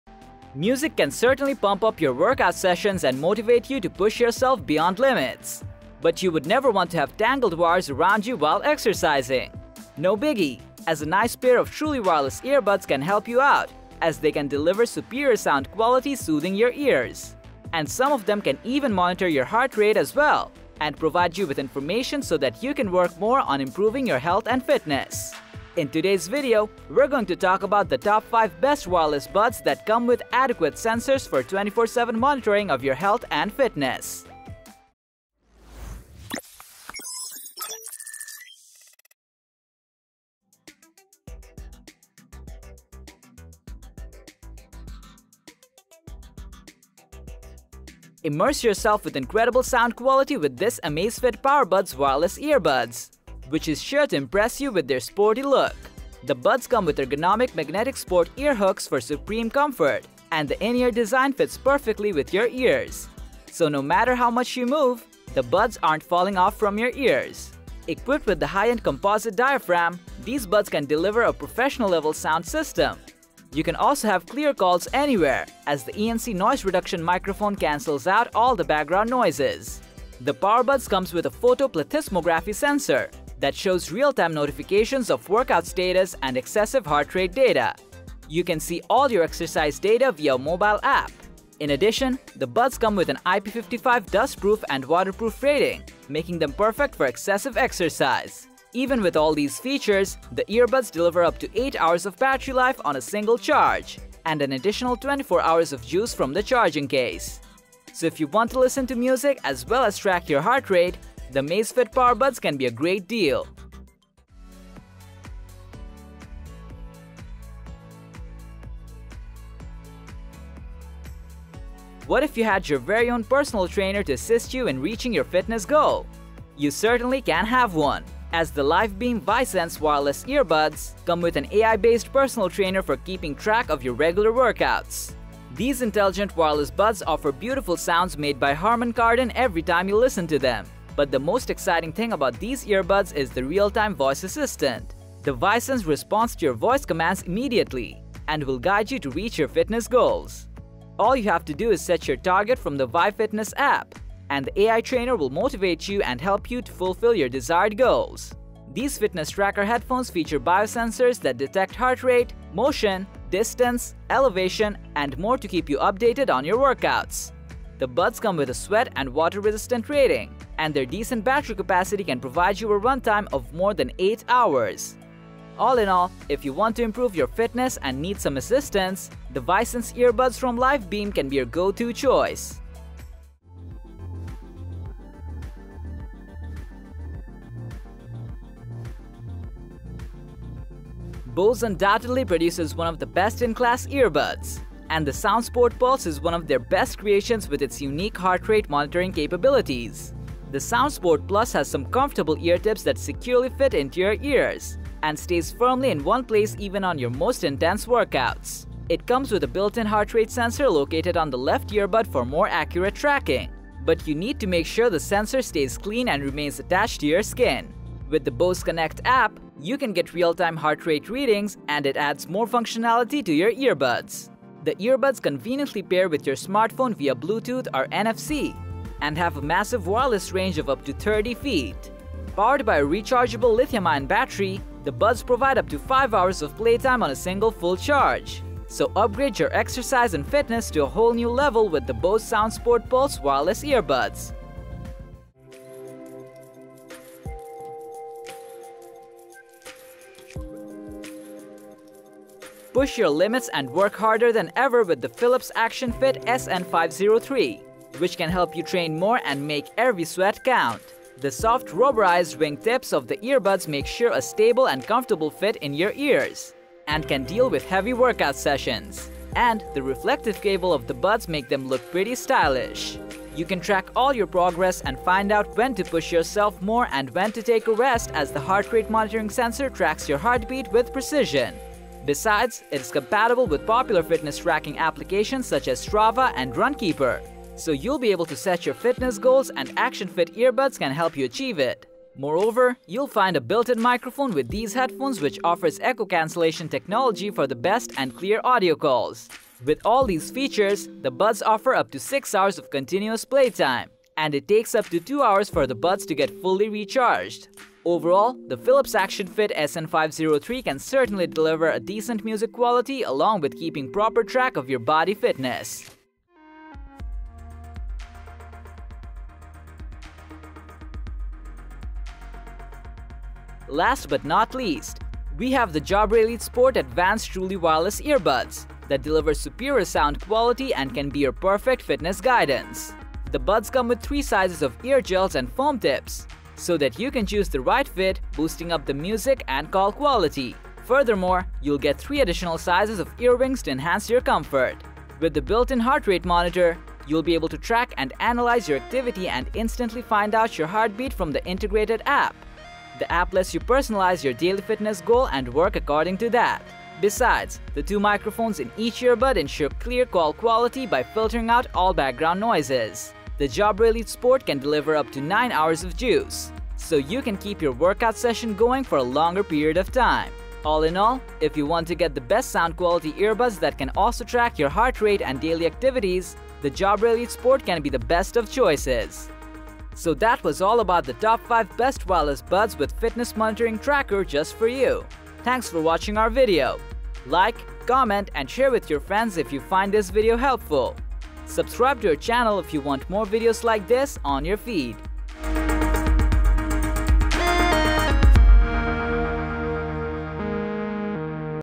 5 Best Wireless Earbuds with Fitness Tracker
Wireless Earbuds can certainly pump up your workout sessions and motivate you to push yourself beyond limits with superior music quality, and some of them even offer Fitness Tracker for mentoring your health, and in this video, we have rounded up the Top 5 Best  Wireless Earbuds with Fitness Tracker that you can get for yourself.

0:00 - Intro

0:40- 5. Amazfit PowerBuds

1:54- 4. LifeBEAM Vi Sense

3:03- 3. Bose SoundSport Pulse

4:05- 2. Philips ActionFit SN503

4:45 - 1. Jabra Elite Sport